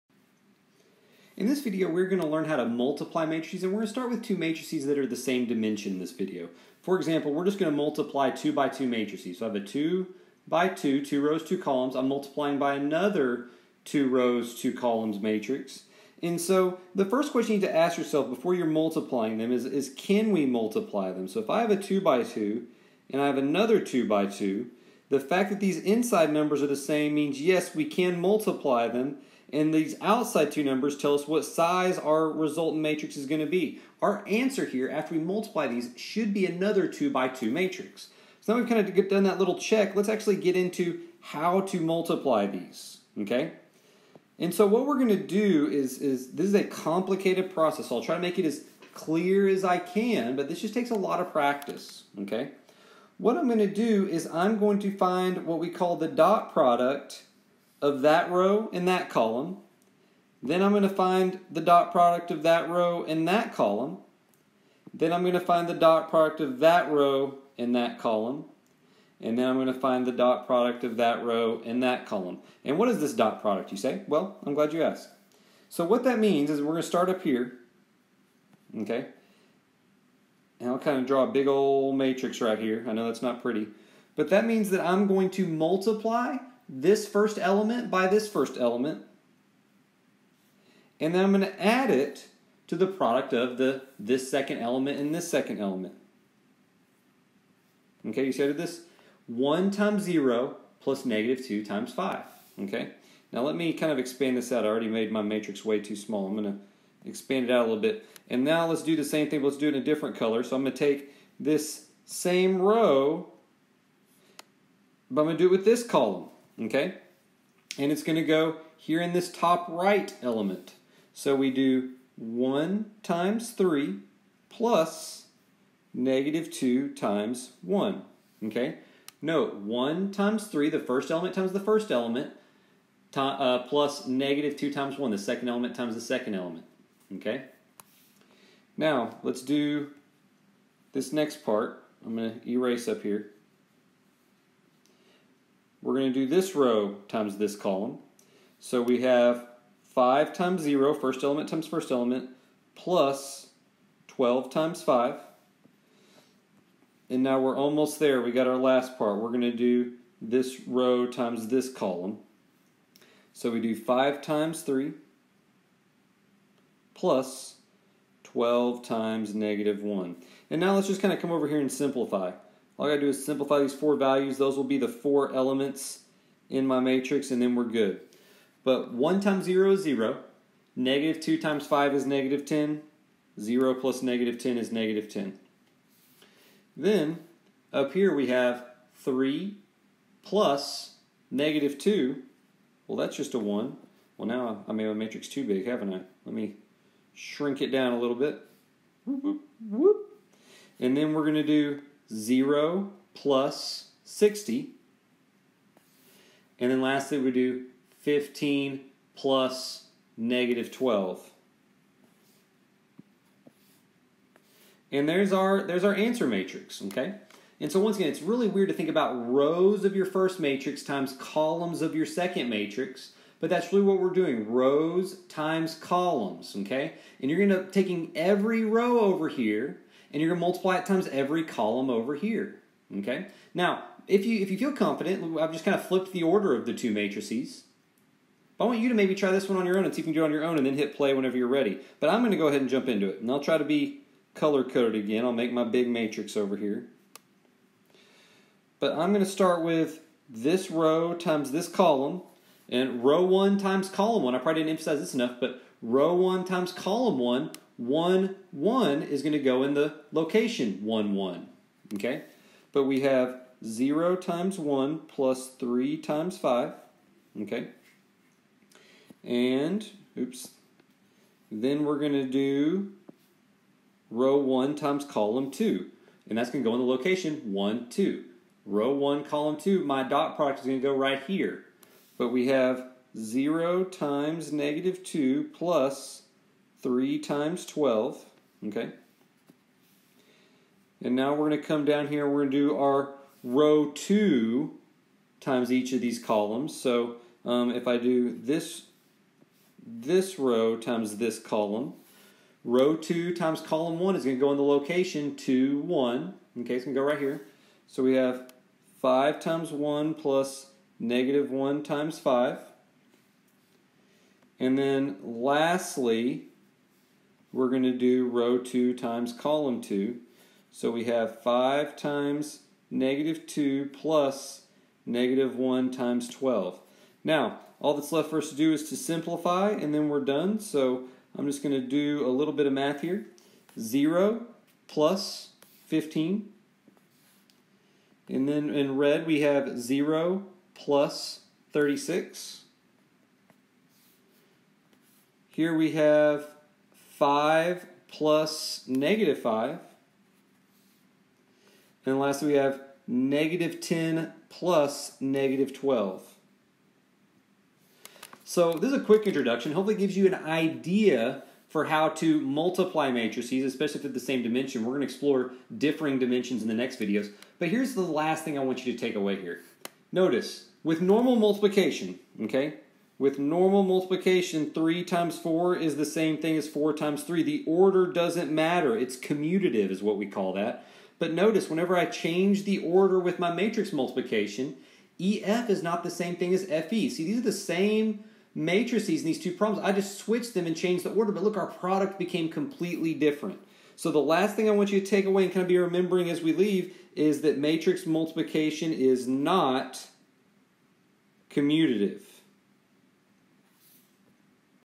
Matrix Multiplication (same dimension)
In this video, we investigate how to multiply matrices that are the same size. We investigate first how to determine if you can even multiply two matrices (depending on the dimension), and then we work out several examples in a step-by-step fashion.

At 2:19, the last red "1" in the first row, second column should be negative.  That throws off the top right element in the matrix.

TIMESTAMPS:
0:00 Determining if you can multiply matrices
0:50 Example 1
3:58 Example 2